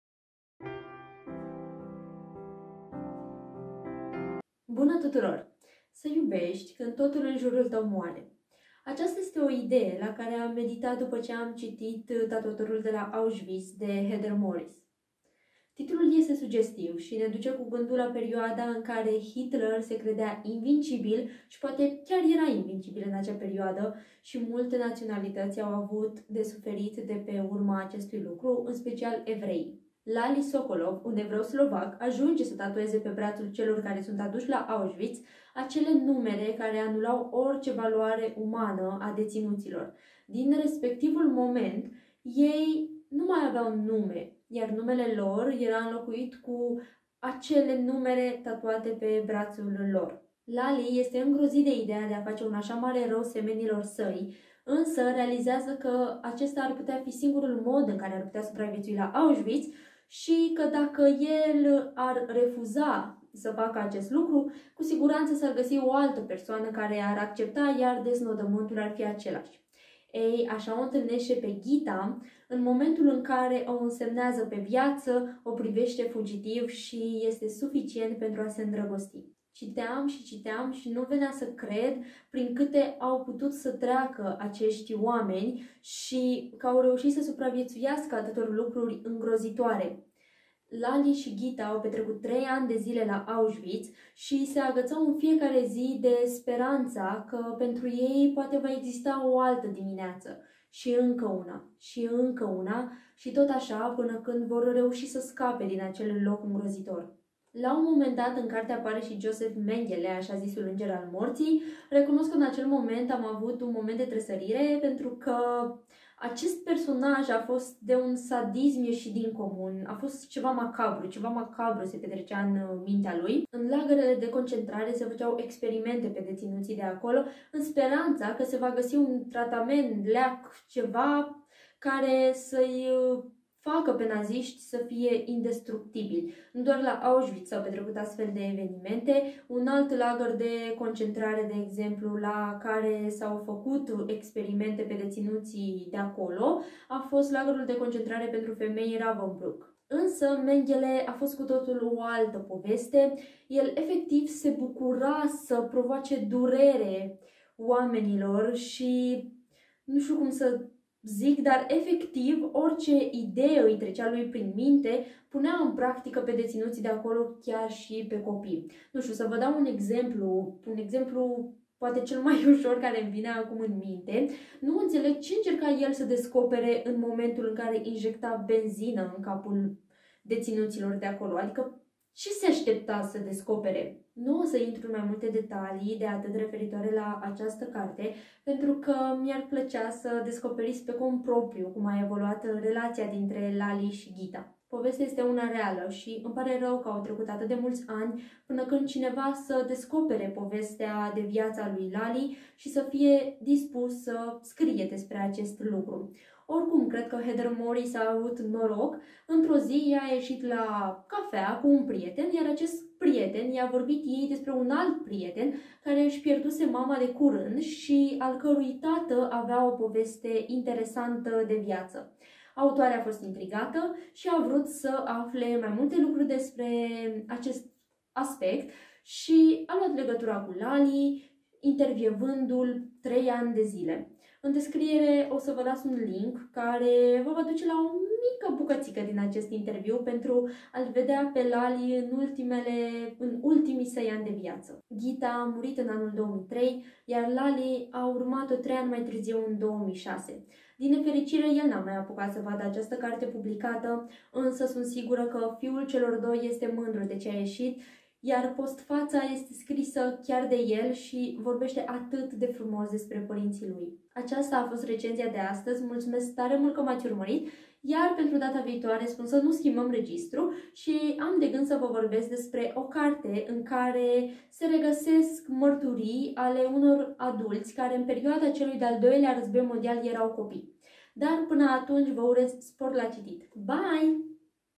Recenzie | Tatuatorul de la Auschwitz
„Tatuatorul de la Auschwitz” este una dintre cărțile mele preferate, mai ales că are la bază fapte reale, ceea ce îi conferă un mare plus pentru mine. Mă atașez de personaje și atunci când știu că acestea sunt plăsmuirea imaginației autorului, însă emoția este mult mai mare atunci când sunt conștientă că personajele în cauză chiar au existat și că au trecut prin toate acele întâmplări dezvăluite în carte.

Vă las aici link-ul, așa cum v-am spus și în clip, cu o fărâmă din interviul cu Lale